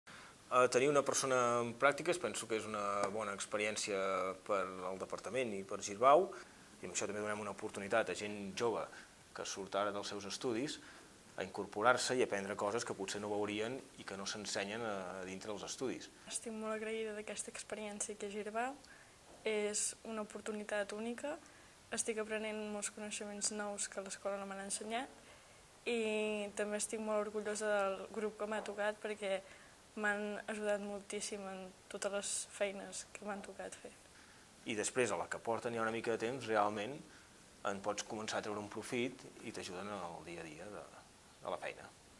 Girbau Talks - Gil Riera y Paula Gil
Gil Riera y Paula Gil, Departamento IT - "Ofrecemos a jóvenes que finalizan sus estudios la oportunidad de incorporar y aprender cosas que no han visto o no les han enseñado en la escuela"

Gil Riera & Paula Gil, IT department -  "We're also providing an opportunity for young people finishing their studies now to incorpórate and to learn things that they might not see and don't get taught in school"